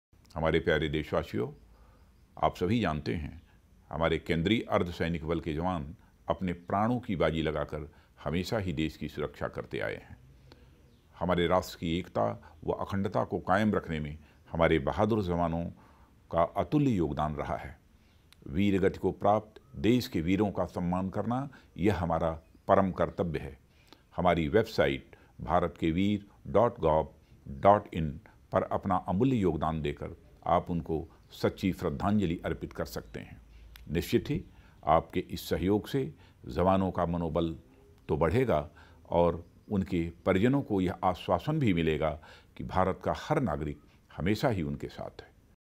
HM Shri Rajnath Singh appeals people to support families of martyrs through Bharat Ke Veer
Home Minister Shri Rajnath Singh appeals people to support families of martyrs through Bharat Ke Veer initiative. Throught BharatKeVeer people can directly contribute to families of Jawans who sacrificed their life in line of duty. Contribution can also be done in Bharat Ke Veer corpus. A maximum of Rs.15 Lacs can be contributed for families of one Jawan in BharatKeVeer initiative.